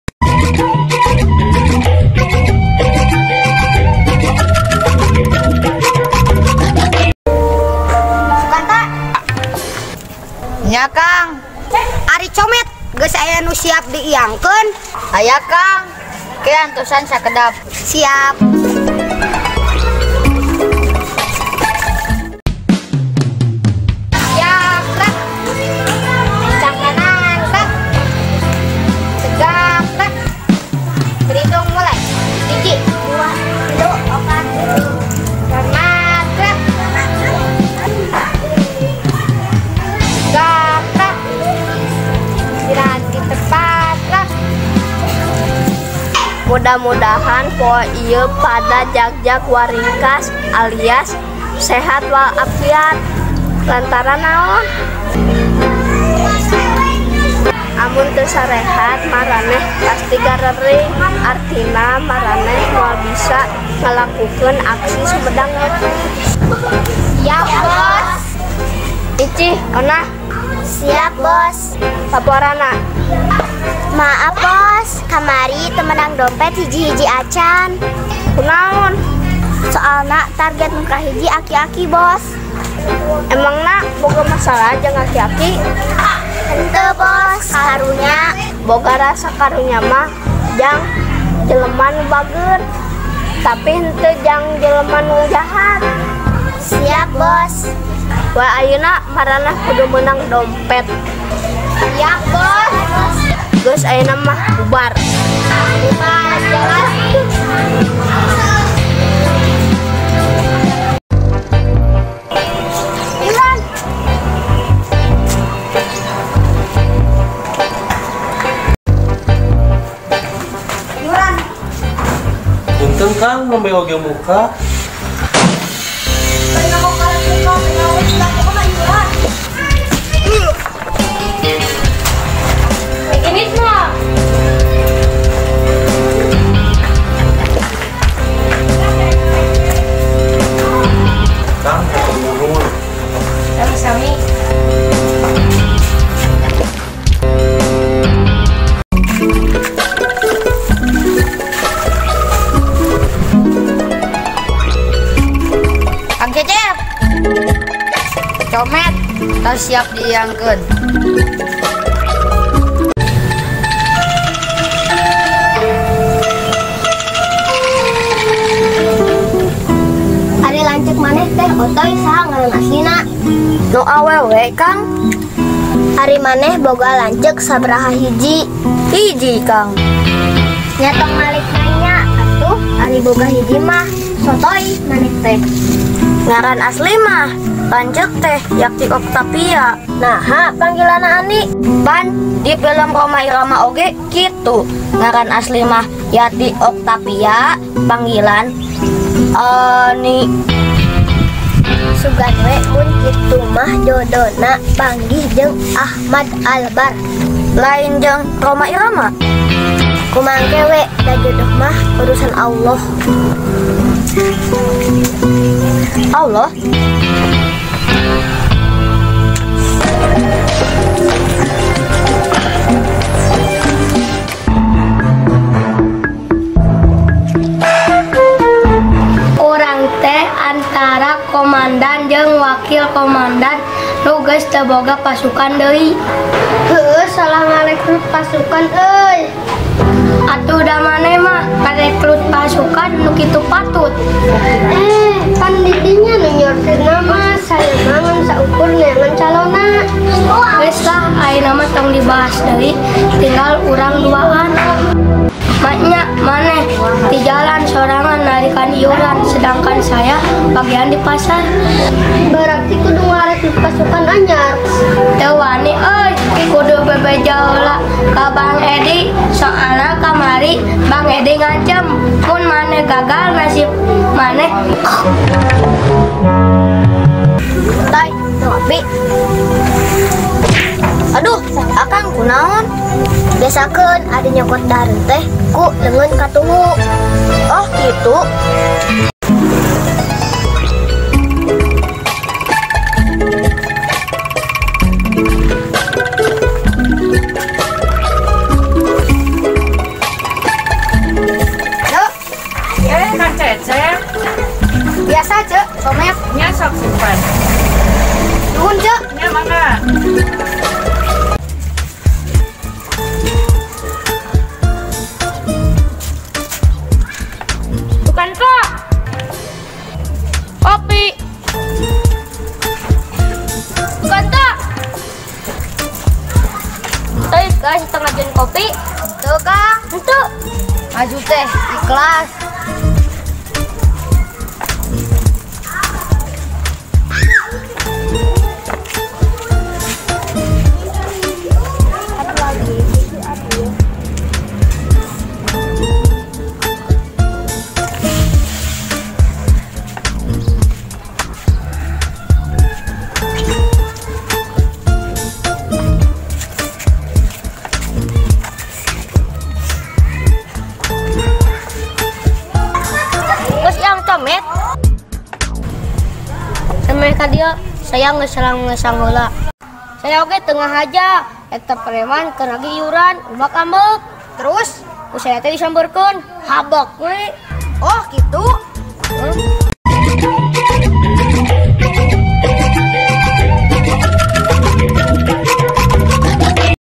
PARODI PREMAN PENSIUN 5 | UJANG KEMBALI SALAM OLAH RAGA
Parodi atau guyonan dalam konten Preman Pensiun kembali hadir. Semoga dulur-dulur merasa terhibur. Cag ah...